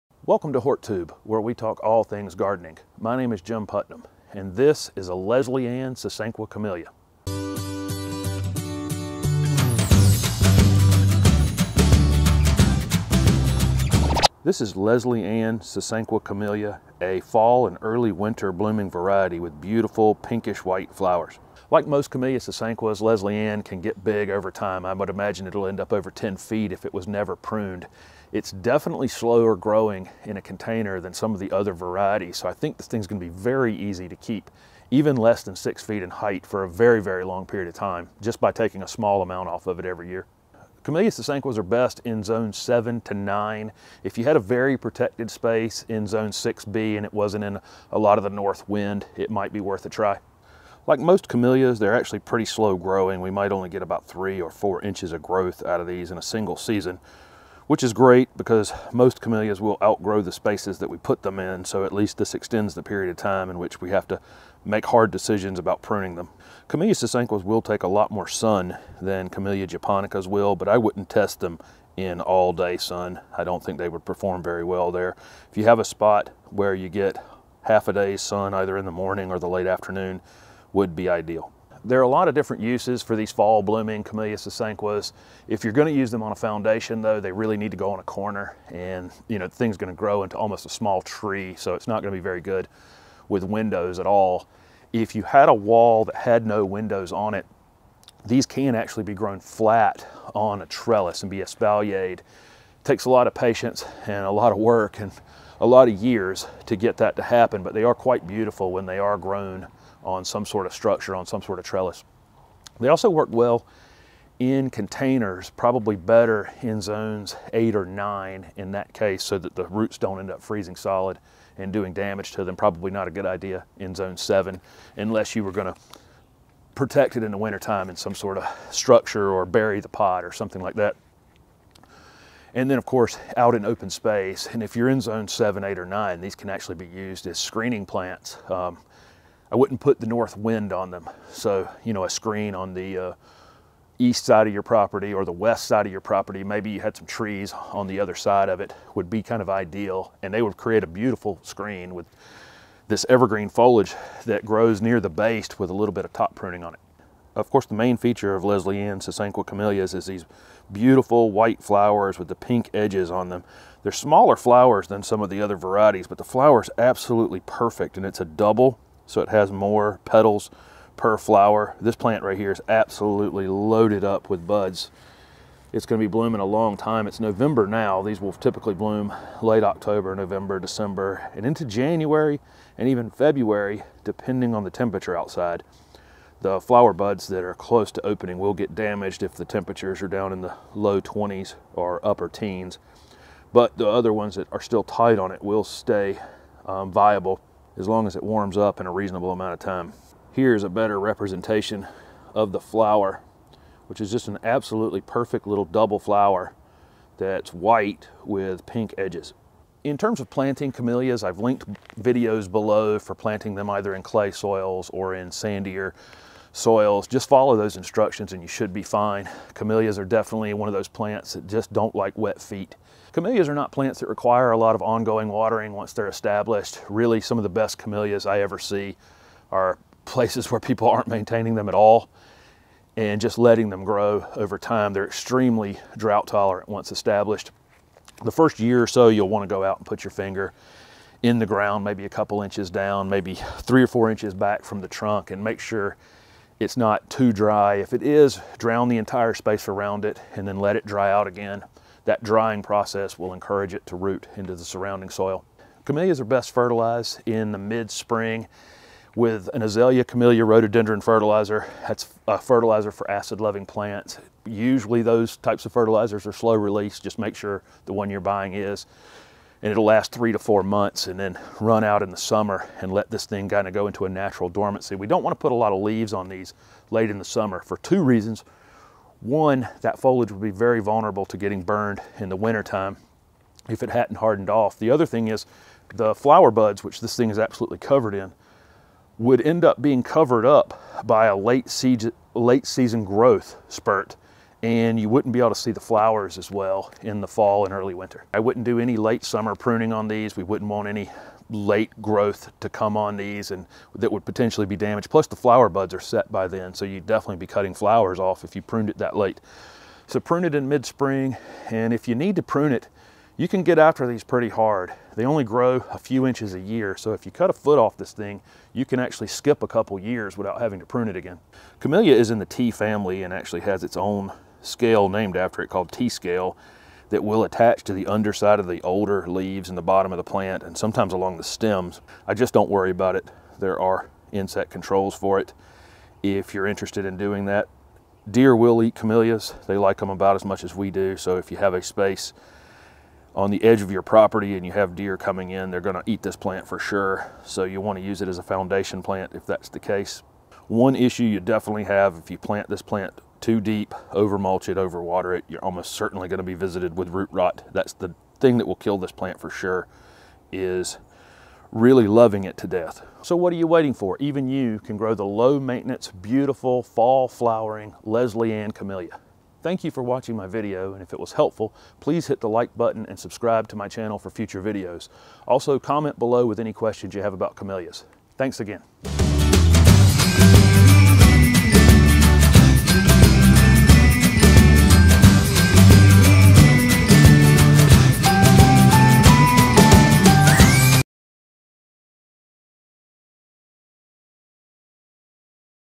How to grow Leslie Ann Sasanqua Camellia with a detailed description
This video is a detailed description of Leslie Ann Sasanqua Camellia. Leslie Ann is a fall and early winter flowering Camellia with white flowers and pink edges. It is evergreen and can grow very large over time. Leslie Ann is best used on the corner of a foundation or as a screening plant in part-shade.

Camellia Sasanqua 'Leslie Ann'
'Leslie Ann' Camellia

Jim has over 30 years experience in the green industry and is the current owner of Earthworks Nursery.